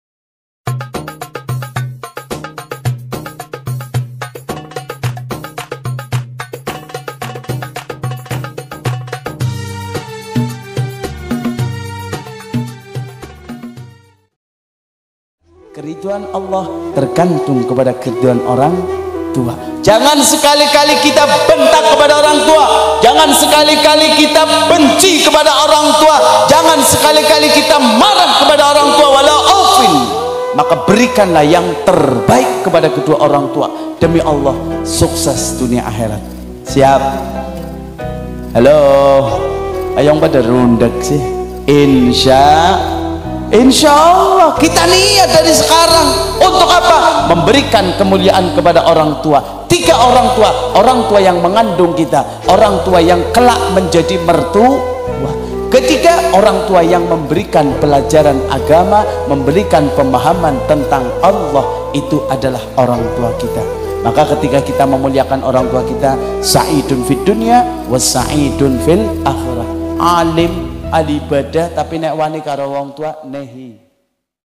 CERAMAH HABIB BADAR AL HABSYI BUAT ORANG SAMPAI MENANGIS... CINTA KEDUA ORANG TUA KITA
Majlis maulidur rosul tegal